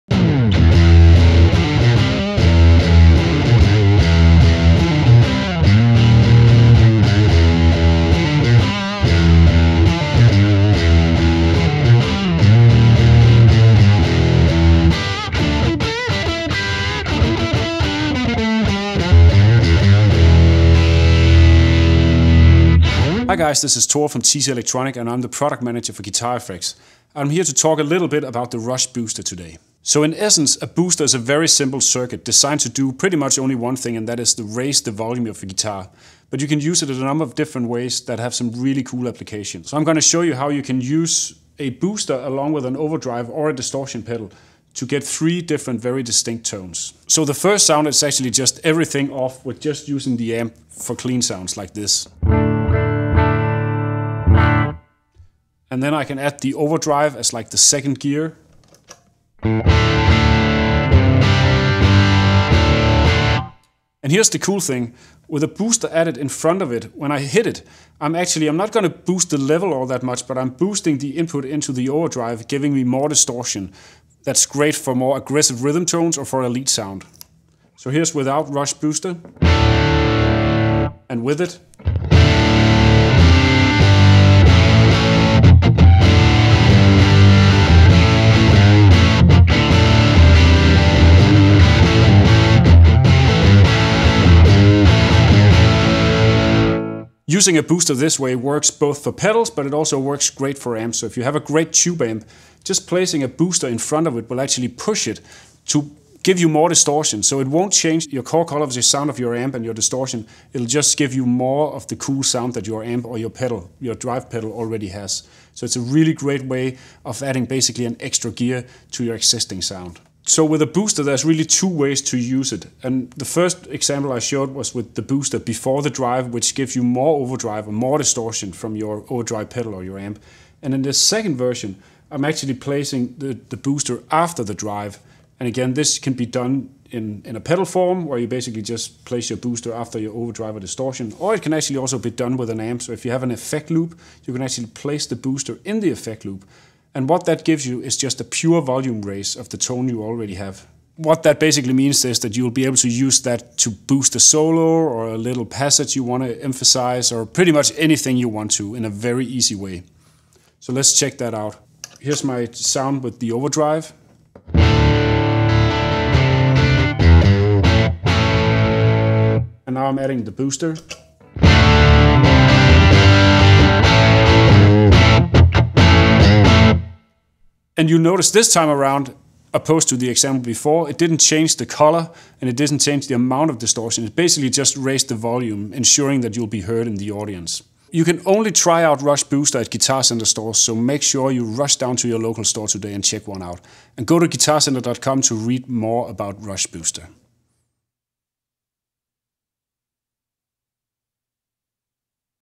Tore Mogensen Demos the TC Electronic Rush Booster Effect Pedal
Rush Booster lets you cut through the clutter and take center stage with one single stomp. Its all-analog circuit offers up a whooping 20 dBs of super-transparent clean boost, perfect for solos or beefed up rhythms. All of this is housed in a built-to-last enclosure with true bypass and at a price point so low it’s sure to set off a certified tonal adrenaline rush!